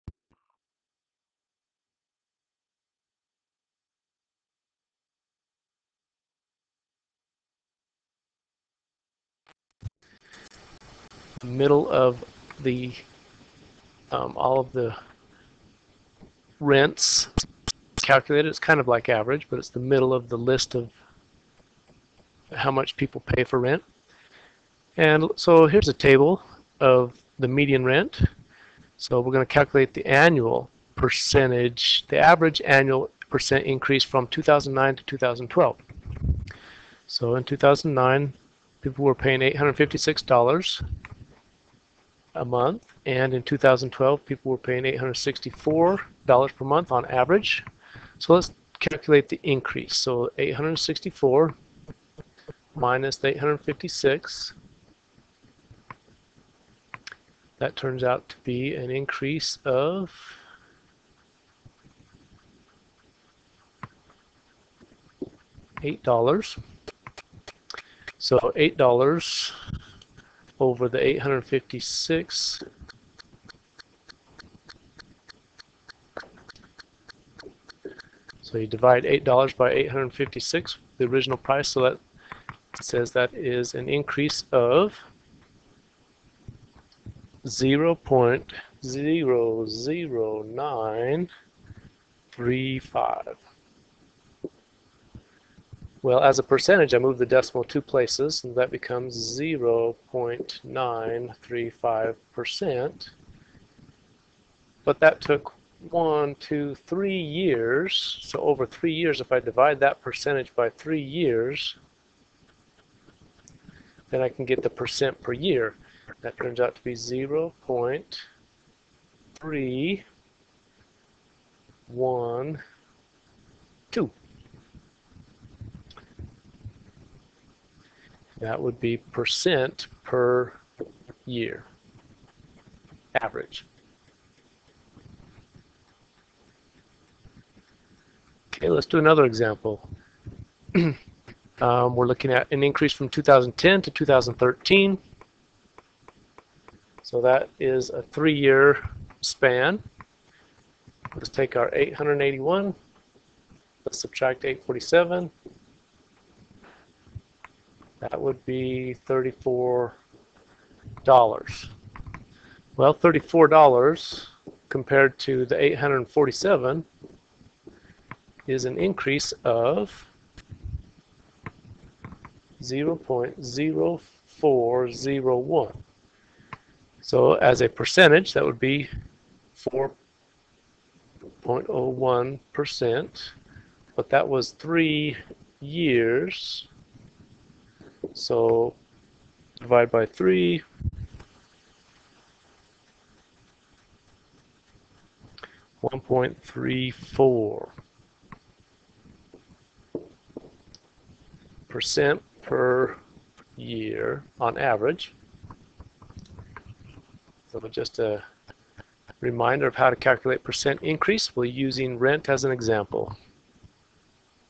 Percent Increase rent video
Calculating percent increase using rent as an example